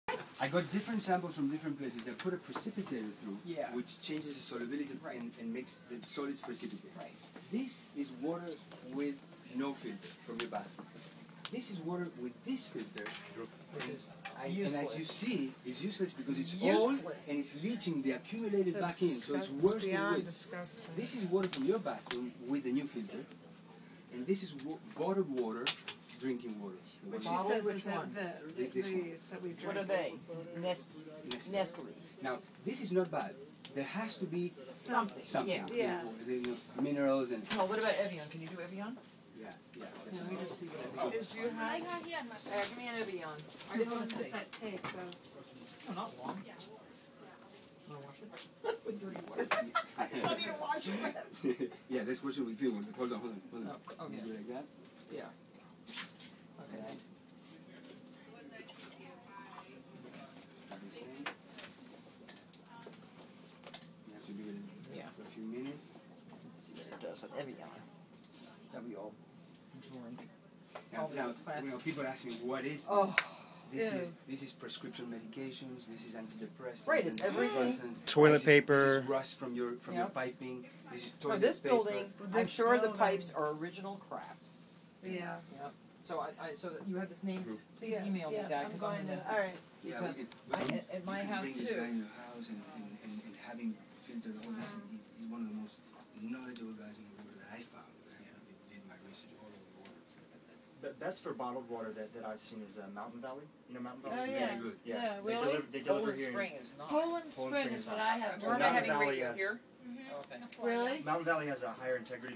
Testing Martha Stewart's Waters
When I went to The Martha Stewart Show I took a precipitator to test the water at the studio. Watch me showing Martha Stewart the condition of the water from different water faucets there.